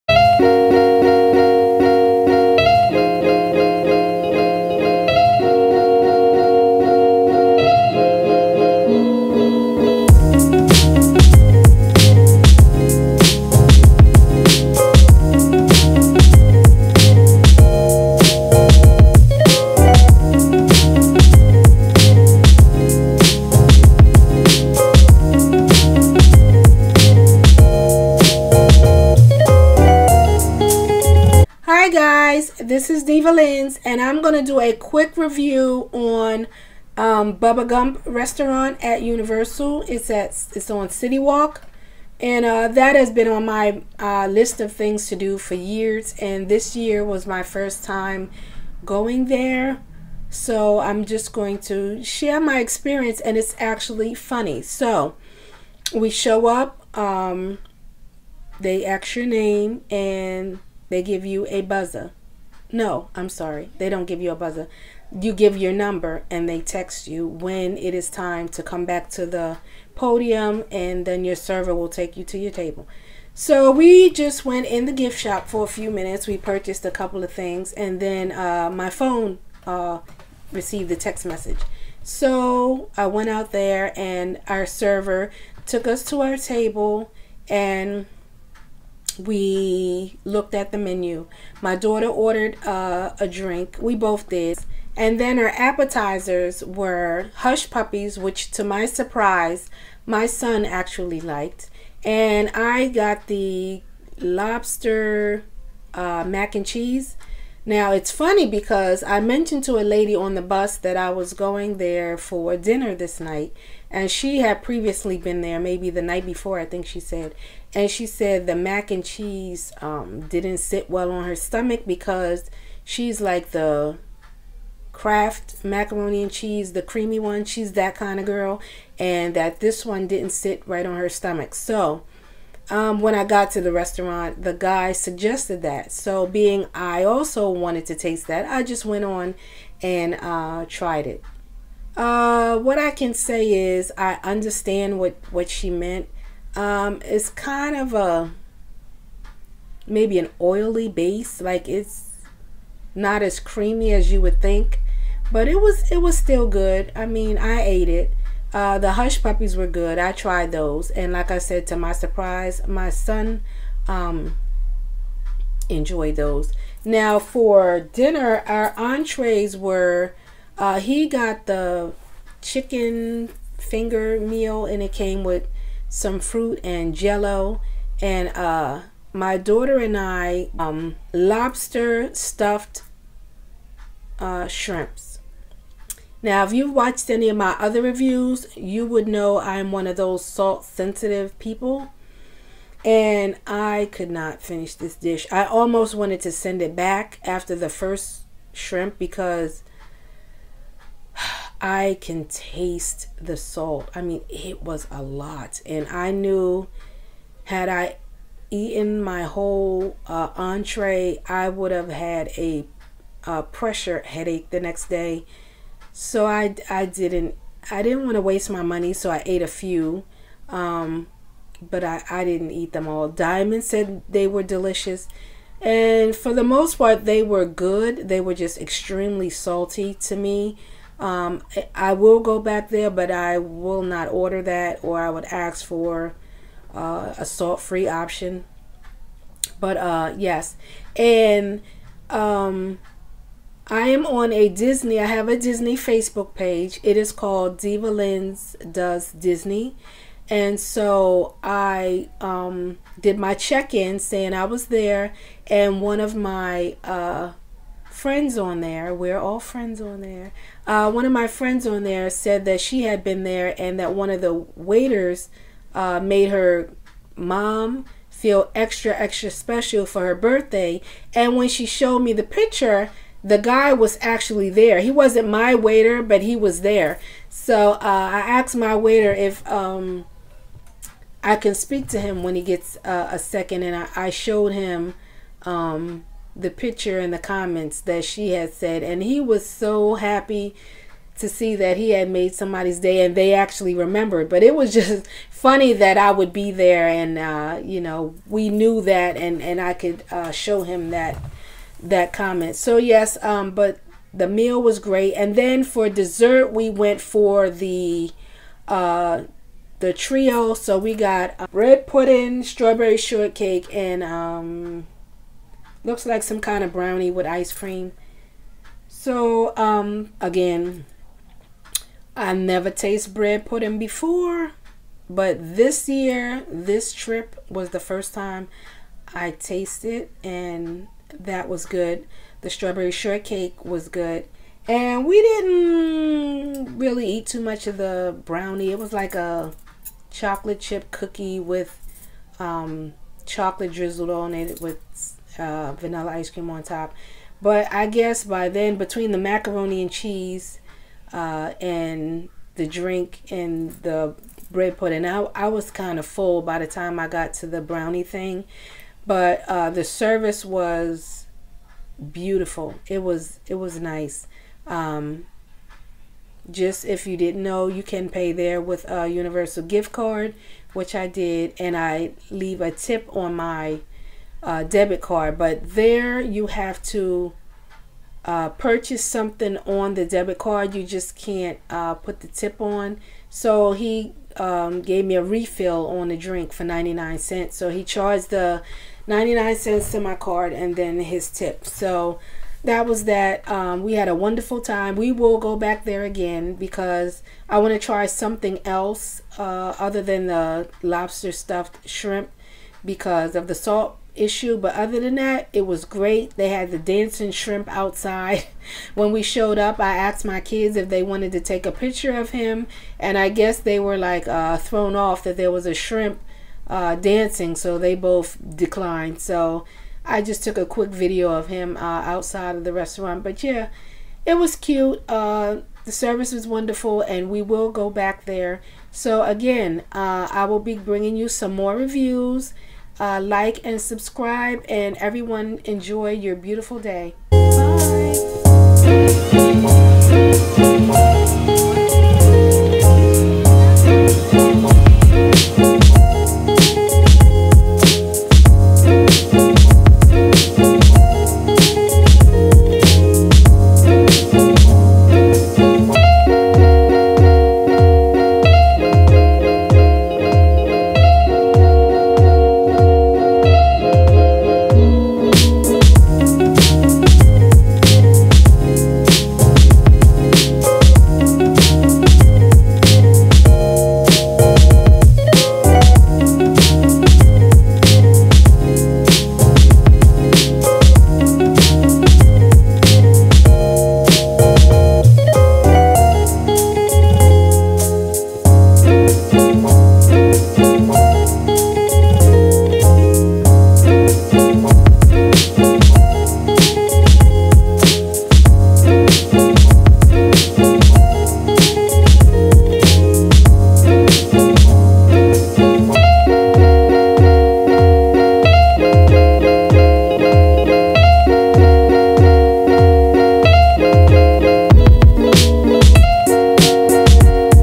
Bubba Gump at City Walk in Orlando,FL
This video is about our dinner at Bubba Gump in Universal's City Walk.     The food was served quickly and for the most part was good. The dessert was to die for.